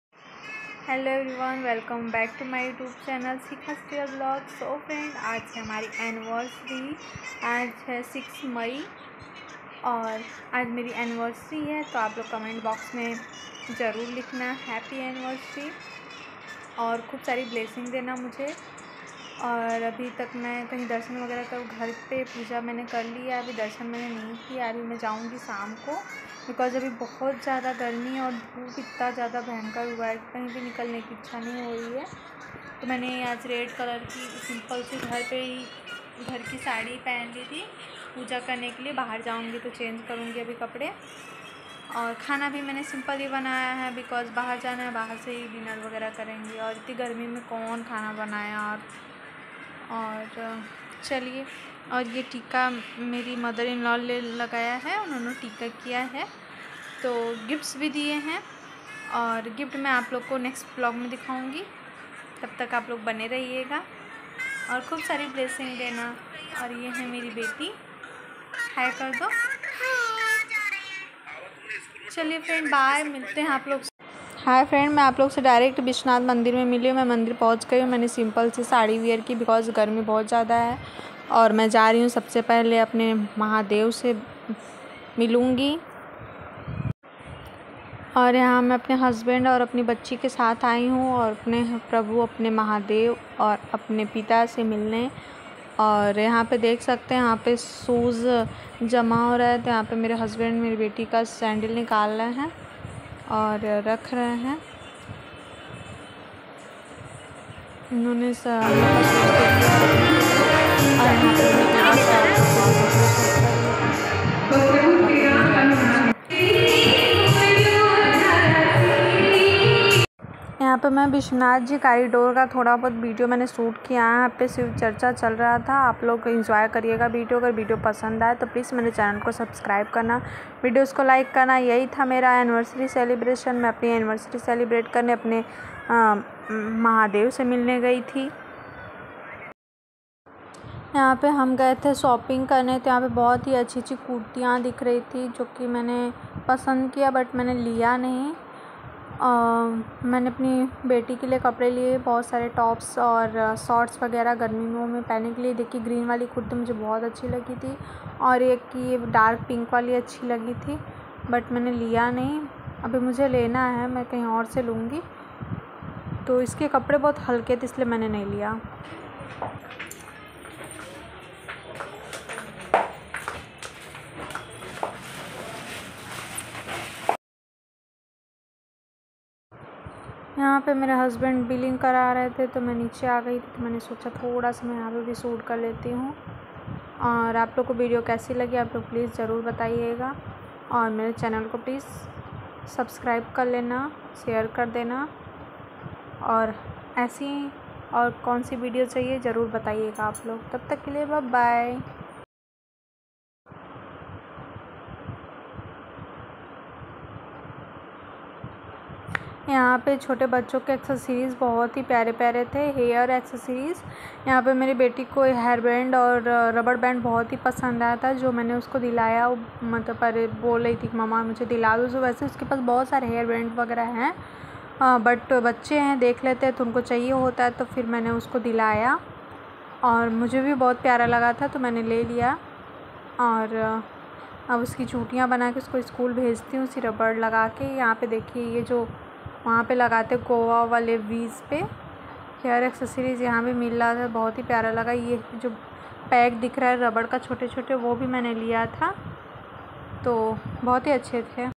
annivarsary pr gye vishwanath ji ka darshan krne ।। Kiya mall me shopping bhut sundr njara temple me
Annivarsary par gye vishwanath ji ka darshan krne Kiya mall se shopping।।bhut sundr njara h